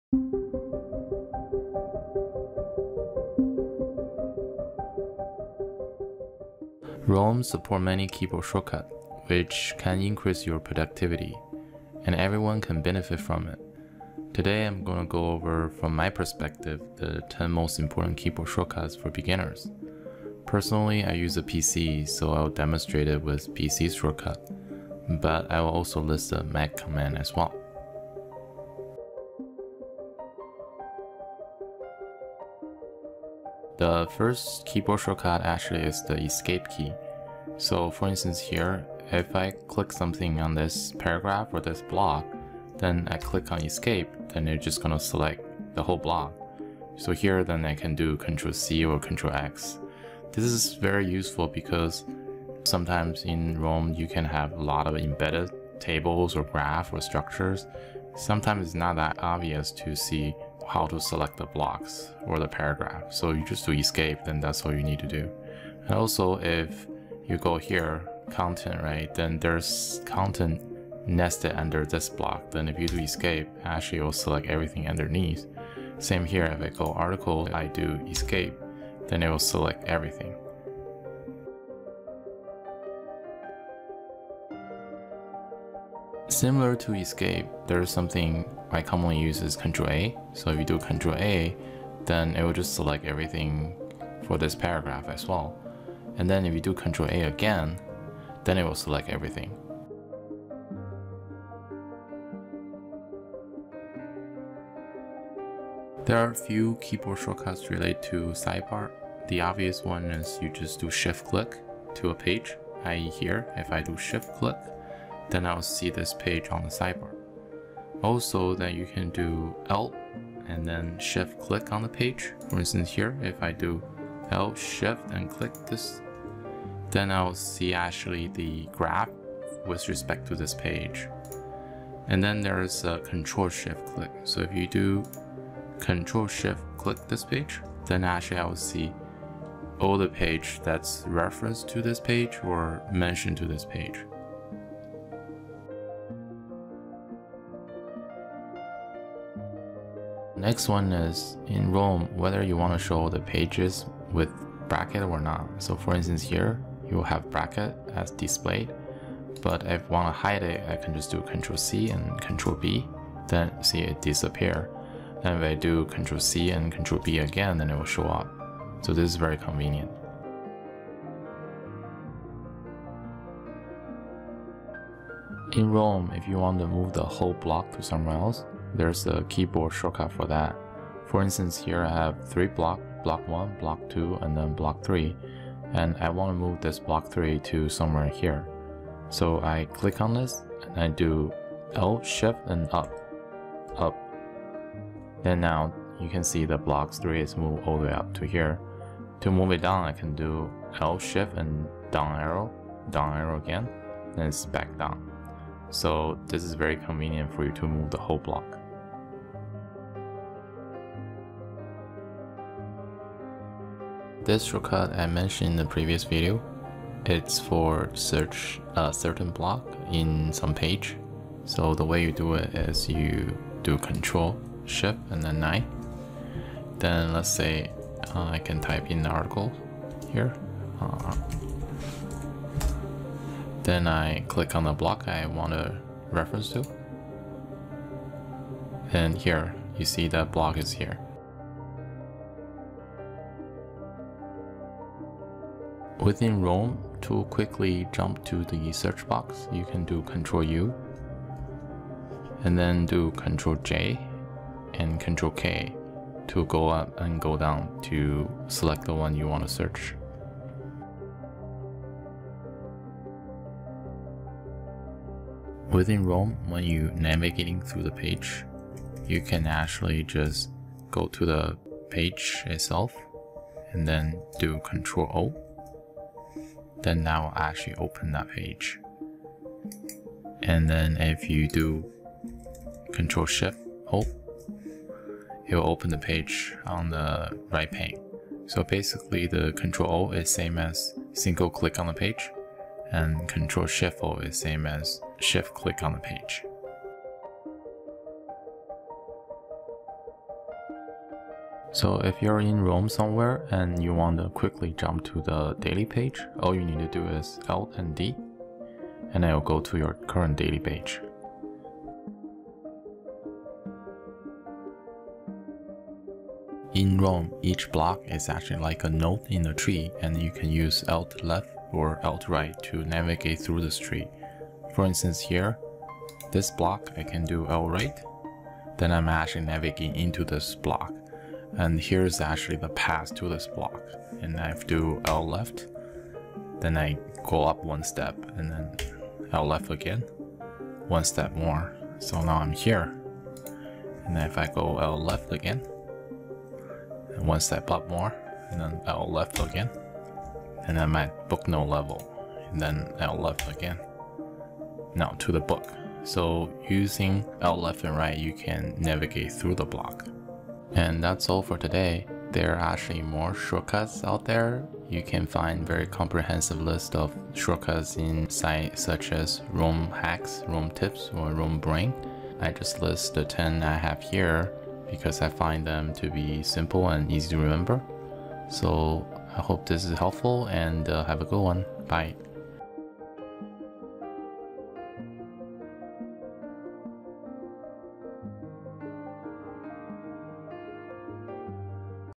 Top 10 Roam Shortcuts for Beginners
10 simple keyboard shortcuts you should know to make Roam more productive.

To see a more comprehensive list of Roam keyboard shortcuts, refer to below:

- 1. ESC key to select blocks
- 2. Ctrl-A & Ctrl-A twice  to select blocks
- 3. Sidebar Controls
    - Windows: Shift-Click, Alt-Shift-Click, Ctrl-Shift-Click
    - Mac: Shift-Click, Opt-Shift-Click, Cmd⌘-Shift-Click
- 4. Toggle Page Brackets View with Ctrl-C, Ctrl-B
- 5. Move Block Up & Down
    - Windows: Alt-Shift-Up
    - Mac:  Cmd⌘-Shift-Up, Cmd⌘-Shift-Down
- 6. Search Blocks through Page with Ctrl-Shift-9
- 7. Jump to Search Box
    - Windows:  Ctrl-U, Ctrl-J, Ctrl-K
    - Mac: Cmd⌘-U, Ctrl-J, Ctrl-K
- 8. Open Page &: Sidebar with Ctrl-O, Ctrl-Shift-O
- 9. Jump to Daily Page
    - Windows: Alt-d
    - Mac: Ctrl-Shift-d
- 10. Navigations through Blocks
    - Windows:  Alt-Left, Alt-Right
    - Mac: Cmd⌘-,    Cmd⌘-.